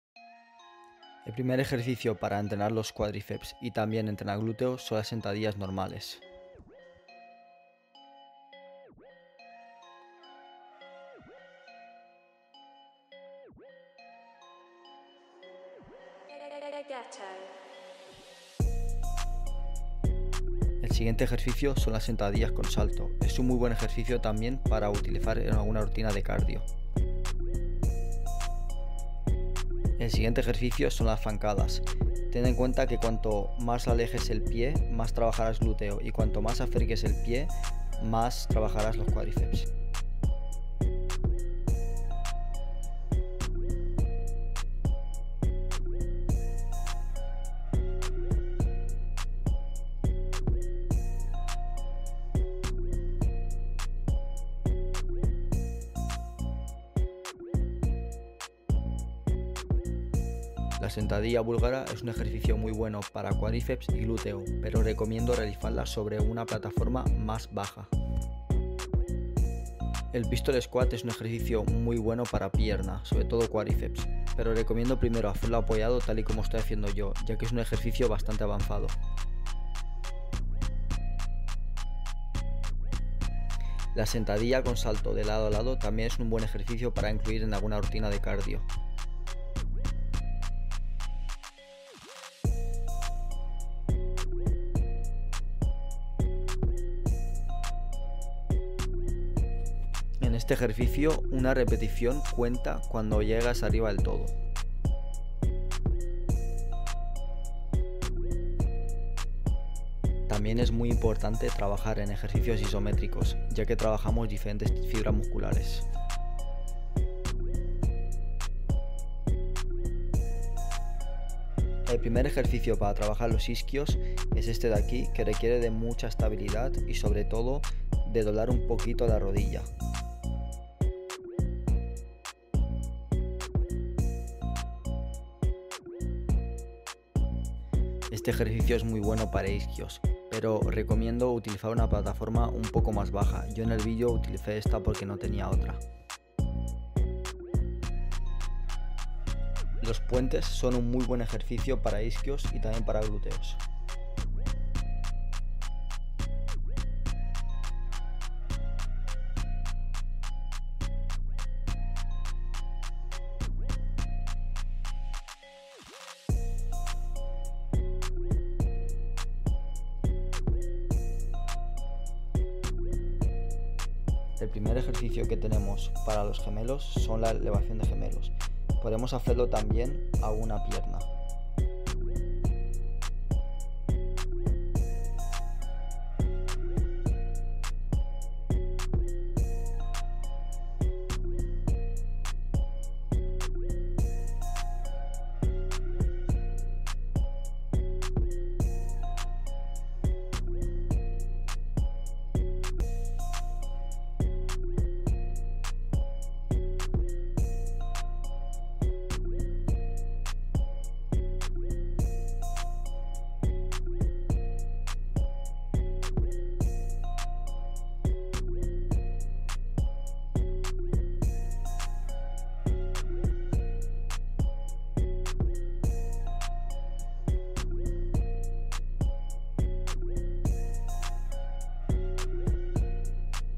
Como empezar calistenia🦁 ( parte 2 ) - Como entrenar piernas
Sabías que puedes trabajar la pierna completa solo utilzando tu propio peso? si no es así, ahora lo sabes, espero que te guste este vídeo de esta serie sobre como empezar entrenando la calistenia, no te olvides de mirarte toda la lista completa para poder aprender todos los consejos y tips que tengo para ti, que he ido aprendiendo a lo largo de los 7 años que llevo en la calistenia.